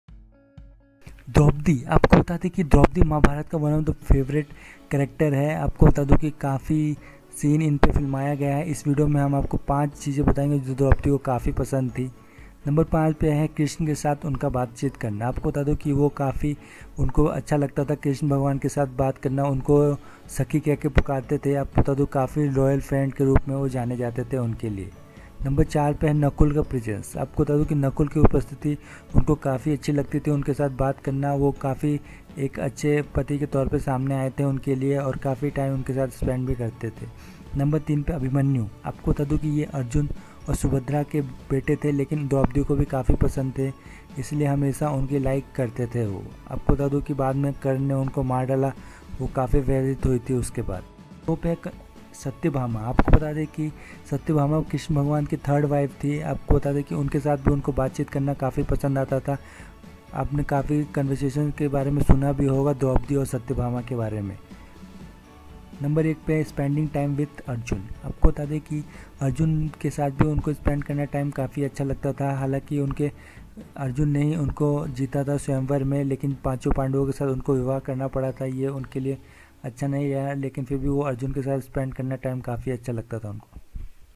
5 Things Draupadi Like in Real Life | Shaheer Sheikh
5 Things Draupadi like in real life?
5 Talk to Krishna
1 Spending time With Arjun
2 Conversation With Satyabhama
3 Abhimnanyu

4 Nakul presence
Note:If Anyone Found This Post Is Offensive I Am Sorry
Credits:news article,wikipedia,various article
Information & media courtesy